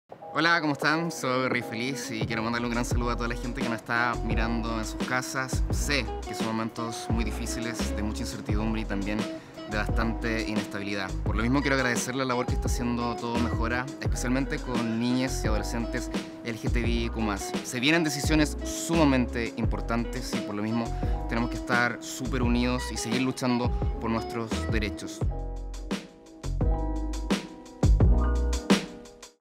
Rey Feliz - Premios Todo Mejora 2022
¡Somos una gran comunidad y cuando nos unimos logramos cosas maravillosas 🌈​!

Como comenta @reyfeliz.wav, debemos seguir trabajando por los derechos de todes, todas y todos 💜

*Recuerdo de PremiosTodoMejora 2021